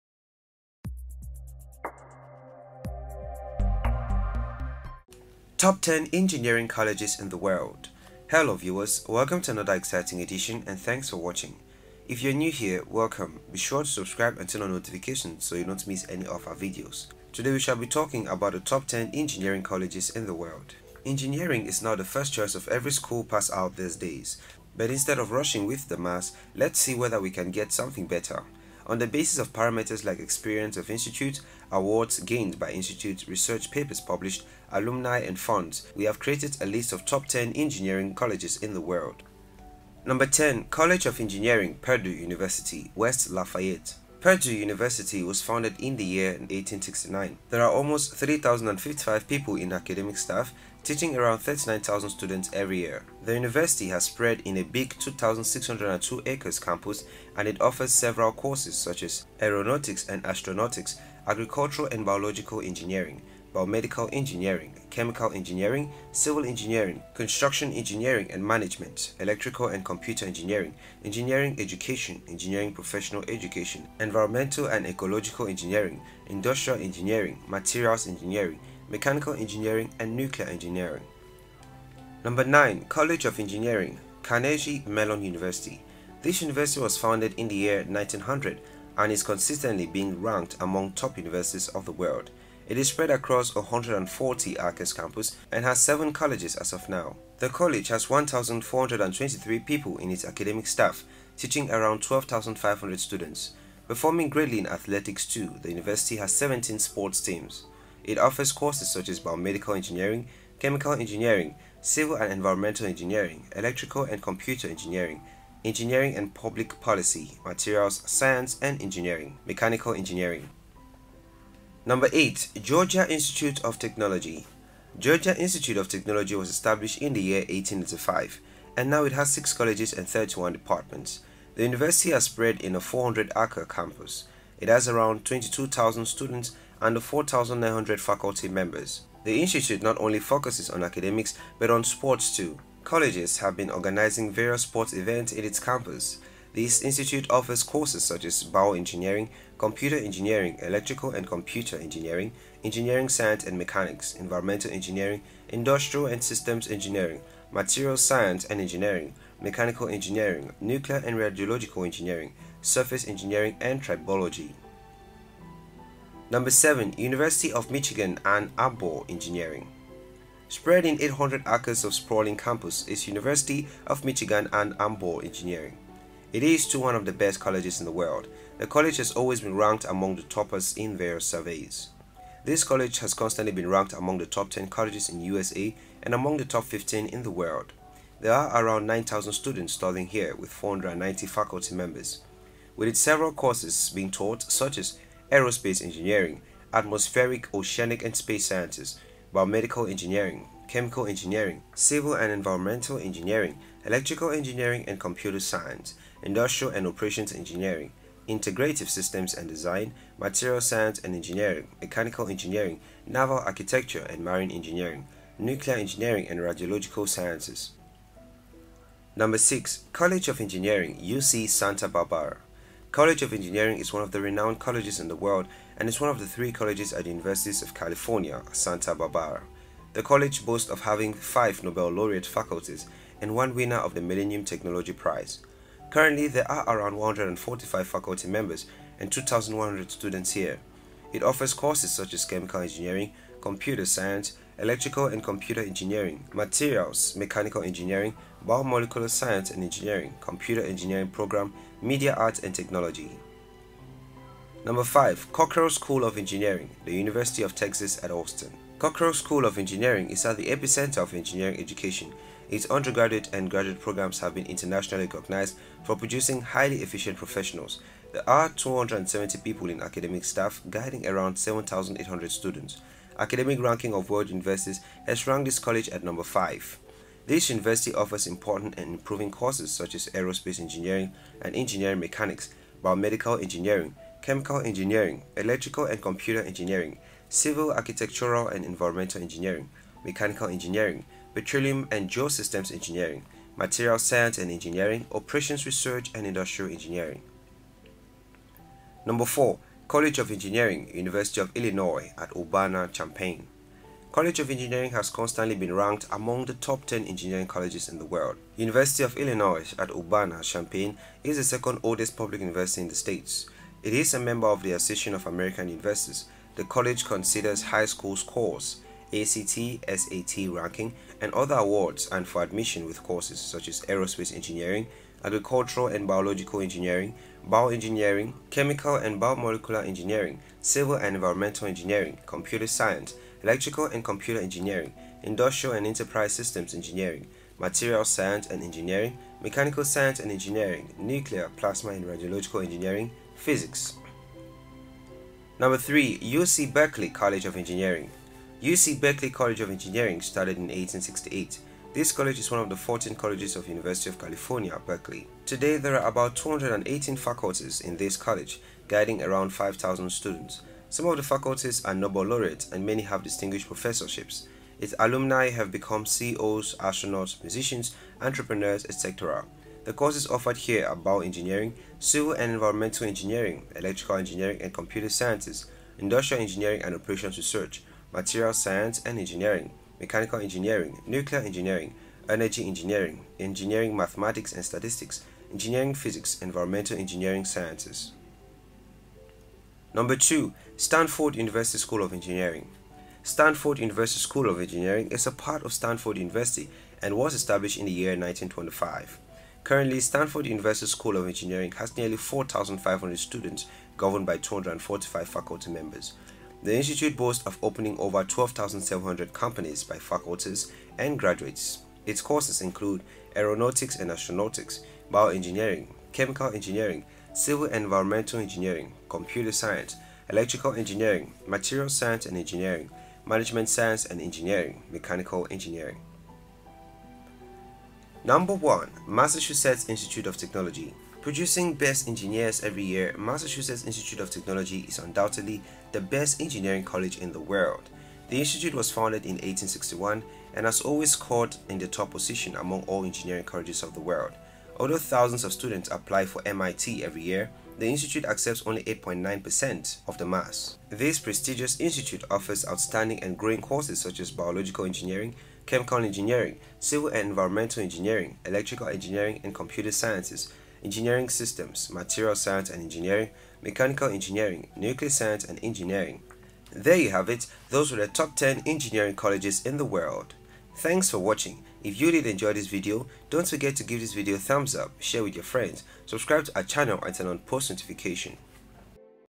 Top 10 Engineering colleges in the world
Top Ten Engineering Colleges in the World

10. College of Engineering-Purdue University, West Lafayette
Purdue University was founded in year 1869. There are almost 3055 people in academic staff teaching around 39000 students every year. The university has spread in a big 2602 acres campus and it offers several courses such as;
• Aeronautics and Astronautics
• Agricultural and Biological Engineering
• Biomedical Engineering
• Chemical Engineering
• Civil Engineering
• Construction Engineering and Management
• Electrical and Computer Engineering
• Engineering Education
• Engineering Professional Education
• Environmental and Ecological Engineering
• Industrial Engineering
• Materials Engineering
• Mechanical Engineering
• Nuclear Engineering

9. College of Engineering – Carnegie Mellon University
This university was founded in 1900 and is consistently been ranked among top universities of the world. It is spread across 140 acres campus and has 7 colleges as of now. The college has 1423 people in its academic staff teaching around 12500 students. Performing greatly in athletics too, the university has 17 sports teams. It offers courses such as;
• Biomedical Engineering
• Chemical Engineering
• Civil & Environmental Engineering
• Electrical & Computer Engineering
• Engineering & Public Policy
• Materials Science & Engineering
• Mechanical Engineering

8. Georgia Institute of Technology
Georgia Institute of Technology was established in year 1885. And now it has six colleges and 31 departments. The university has spread in a 400 acre campus. It has around 22000 students and 4900 faculty members. The institute not only focuses on Academics but on sports too. College has been organizing various sports event in its campus. This institute offers courses such as;
• Bioengineering
• Computer Engineering
• Electrical & Computer Engineering
• Engineering Science & Mechanics
• Environmental Engineering
• Industrial & Systems Engineering
• Materials Science & Engineering
• Mechanical Engineering
• Nuclear & Radiological Engineering
• Surface Engineering & Tribology

7. University of Michigan Ann Arbor Engineering
Spread in 800 acres of sprawling campus is University of Michigan Ann Arbor Engineering. It is too one of the best colleges in the world. The college has always been ranked among the toppers in various surveys. This college has constantly been ranked among top ten colleges in USA and among top 15 colleges in world. There are around 9000 students studying here with 490 faculty members. With its several courses such as;
• Aerospace Engineering
• Atmospheric, Oceanic & Space Sciences
• Biomedical Engineering
• Chemical Engineering
• Civil & Environmental Engineering
• Electrical Engineering & Computer Science
• Industrial & Operations Engineering
• Integrative Systems & Design
• Materials Science & Engineering
• Mechanical Engineering
• Naval Architecture & Marine Engineering
• Nuclear Engineering & Radiological Sciences

6. College of Engineering, UC Santa Barbara
College of Engineering is one the renowned colleges in the world and is one of the three colleges at University of California, Santa Barbara. The college boasts of having five Nobel laureate faculties and one winner of the Millennium Technology Prize. Currently, there are around 145 faculty members, and 2100 students here. It offers courses such as;
• Chemical Engineering
• Computer Science
• Electrical & Computer Engineering
• Materials
• Mechanical Engineering
• Biomolecular Science and Engineering
• Computer Engineering Program
• Media Arts & Technology

5. Cockrell School of Engineering – The University of Texas at Austin
Cockrell School of Engineering is at the epicenter of engineering education. Its undergraduate and graduate programs have been internationally recognized for producing highly efficient professionals. There are 270 people in academic staff guiding around 7800 students. Academic Ranking of World Universities has ranked this college at number five. This university offers important and improving courses such as;
• Aerospace Engineering & Engineering Mechanics
• Biomedical Engineering
• Chemical Engineering
• Civil, Architectural & Environmental Engineering
• Mechanical Engineering
• Petroleum &Geosystems Engineering
• Materials Science and Engineering
• Operations Research & Industrial Engineering